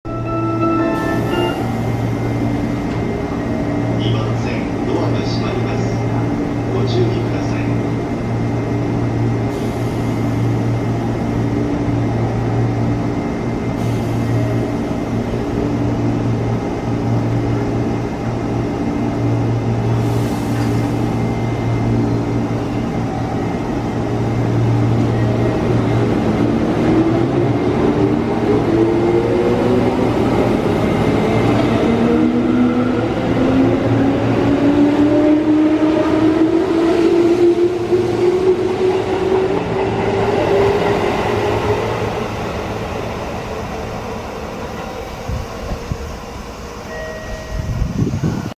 211系1000番台長ナノN323編成 JR中央線普通高尾行き 石和温泉駅発車
2019年(令和元年)8月14日撮影
お盆に撮影した動画なのですが、中央線はやはりそれなりに人が乗っていますね。

※荒らし行為･中傷行為は厳禁です！